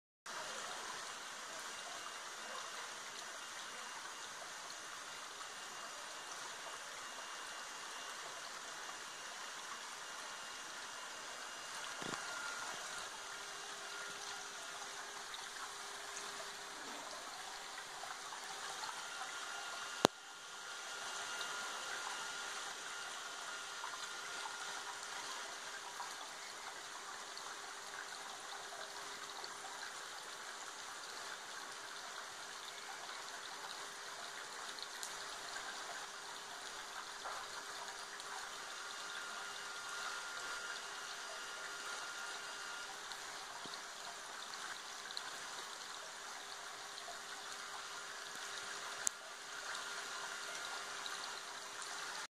Goldfish enjoying the rain and waterfall
New pond: Our fish enjoying the rain and waterfall!